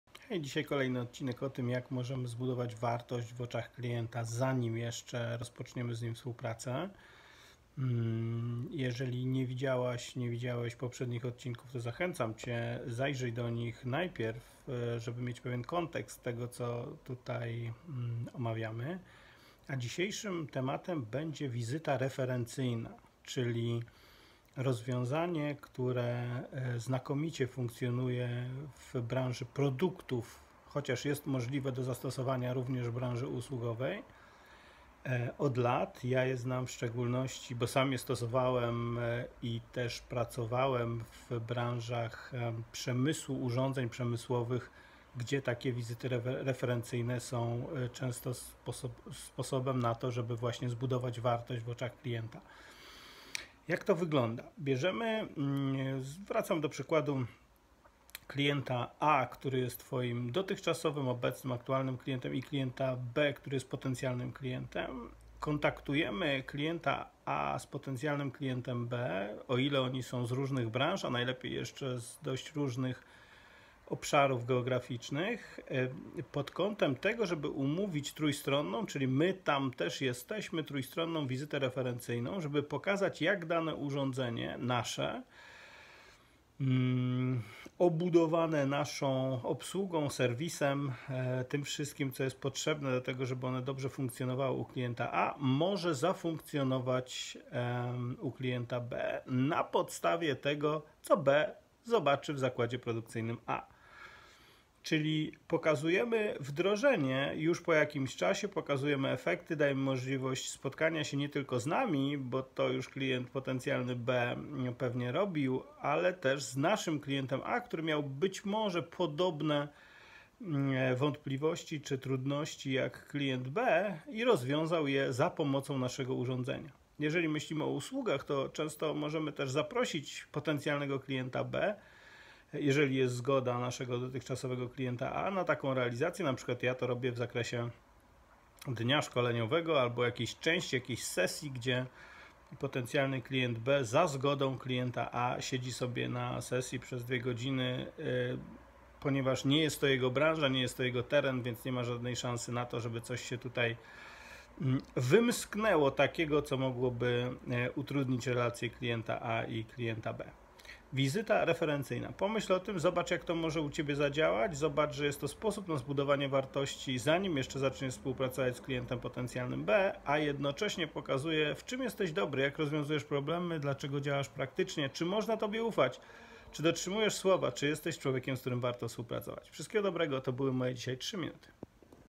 (4) Jak przekonać klienta o naszej wartości - wizyta referencyjna, czyli odc 420 z „Moje 2 minuty”
Jak w temacie - niech jedni klienci pomagają nam zdobywać kolejnych :)